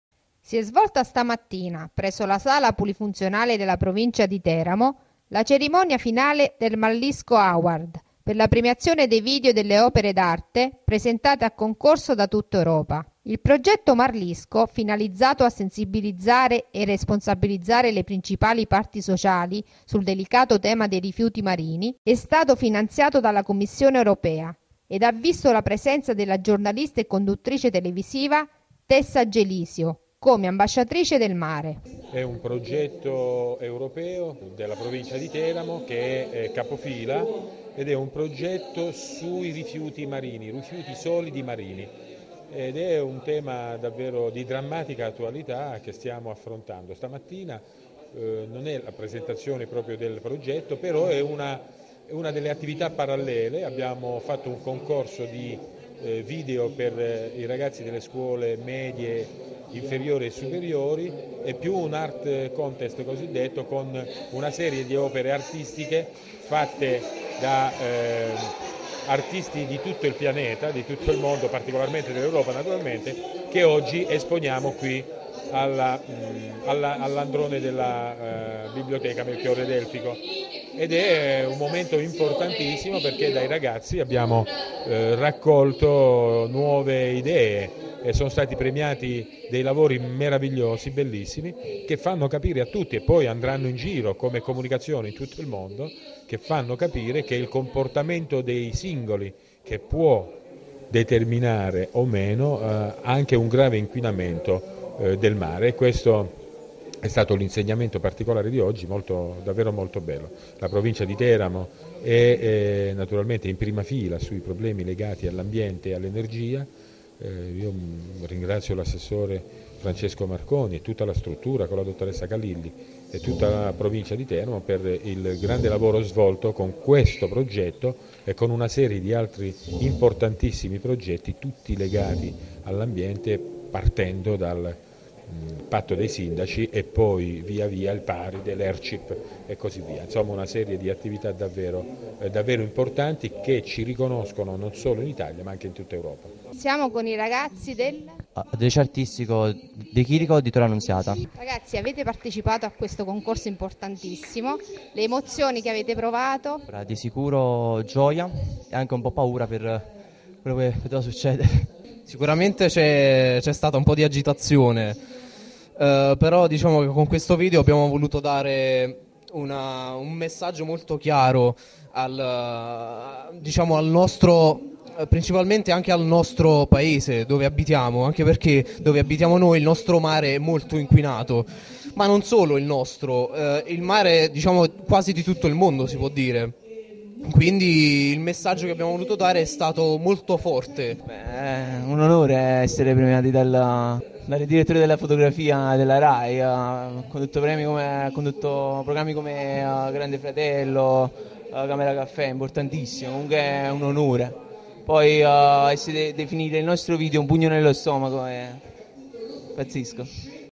Marlisco Award Ceremony
Si e' svolta stamattina,presso la Sala Polifunzionale della Provincia di Teramo,la cerimonia finale del "Marlisco Award" per la premiazione dei video e delle opere d'arte presentate a Concorso da tutta Europa. Il progetto Marlisco ,finalizzato a sensibilizzare e responsabilizzare le principali parti sociali sul delicato tema dei rifiuti marini,e' stato finanziato dalla Commissione Europea ed ha visto la presenza della giornalista e conduttrice televisiva,Tessa Gelisio,come "ambascitrice del mare".Il servizio e le riprese sono di Maria Carannante.